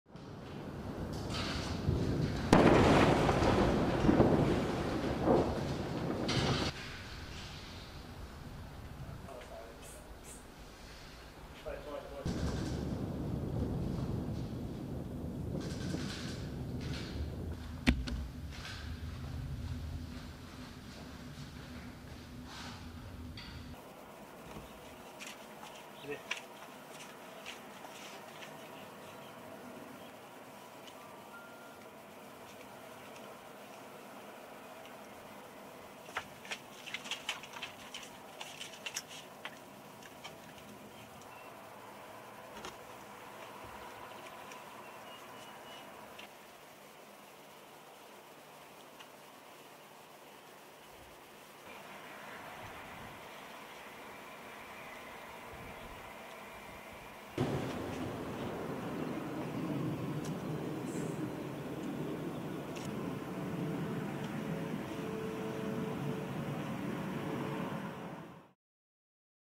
ضربات إسرائيلية على قرى جنوبية لبنانية | AFP
تتصاعد سحب الدخان فوق قرى في جنوب لبنان مع استمرار القصف الإسرائيلي.